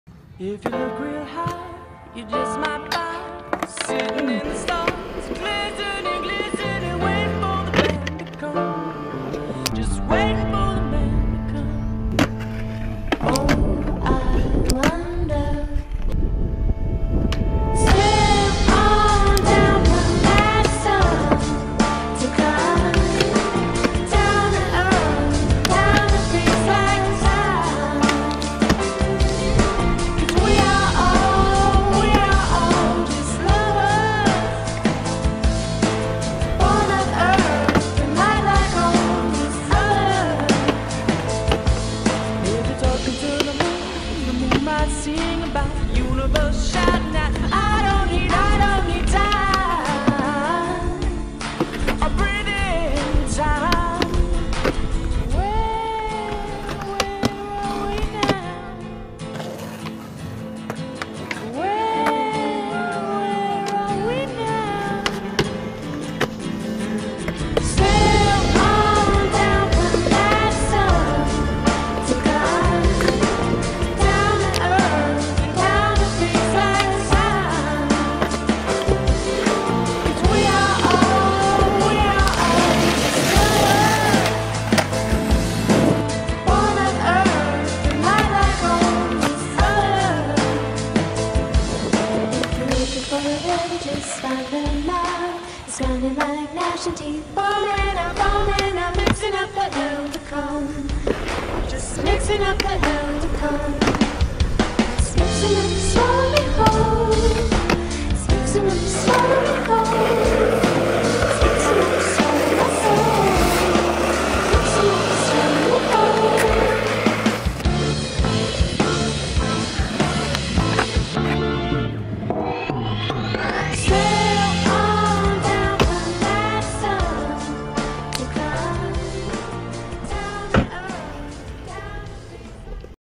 Jake Nafziger-Elmira Skatepark
The clips I've gotten so far at the new park.